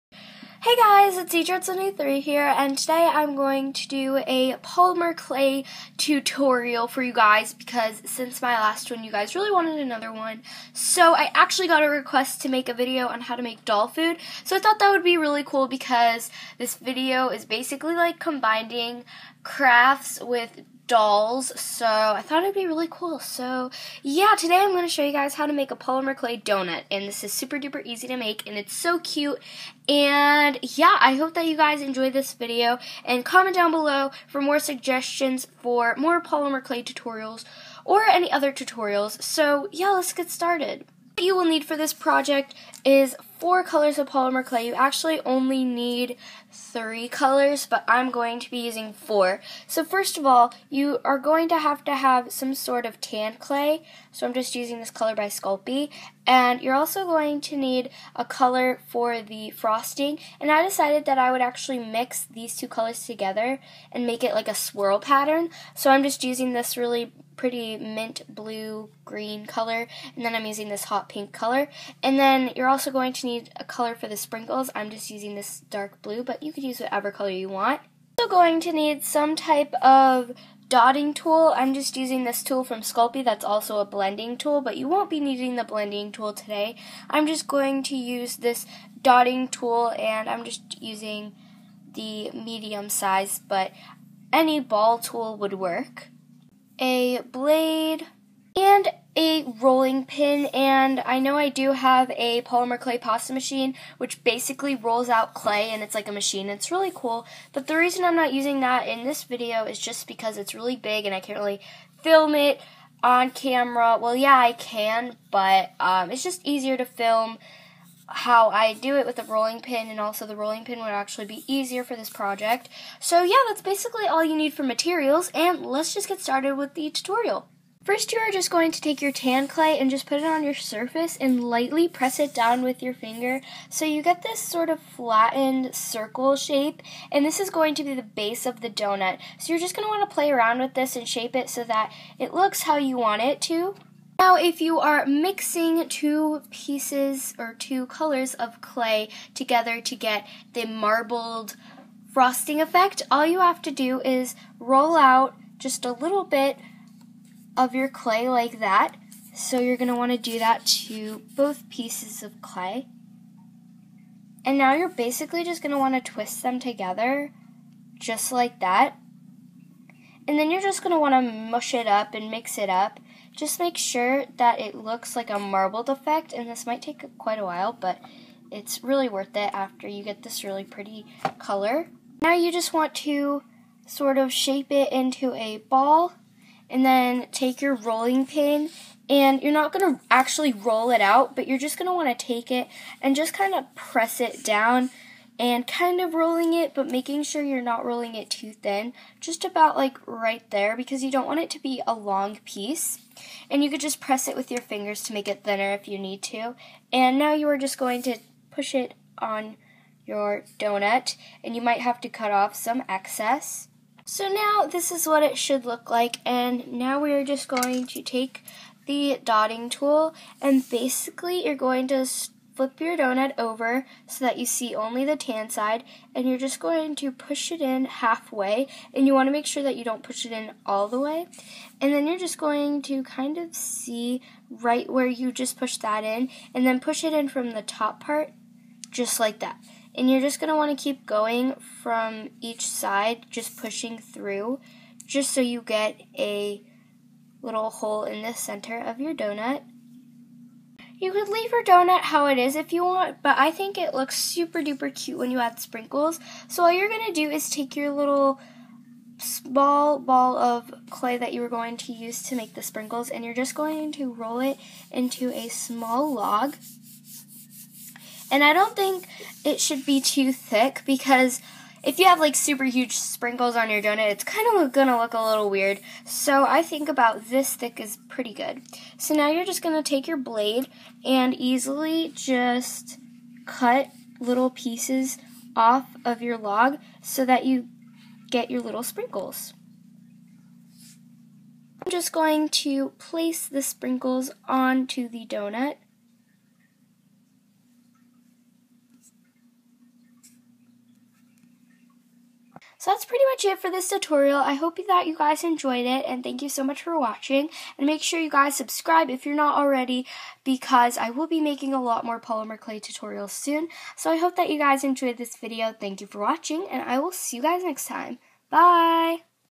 HTM a donut for your AG dolls out of polymer clay!
Read me!

Hey guys, it's me! Duhh. Anyways I'm back with another clay tutorial! I hope you enjoy it! This was requested by NutsforAG! Thanks for requesting!! Comment some more ideas for clay tutorials and you might get a shoutout in my next polymer clay tutorial!